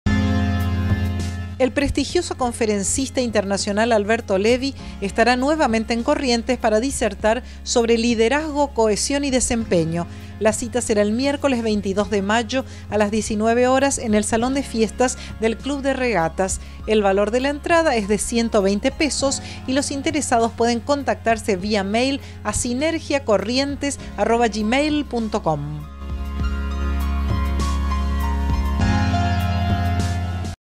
Sinergia Eventos
Nuevamente estará en Corrientes Alberto Levy para dar una charla, imperdible . . . .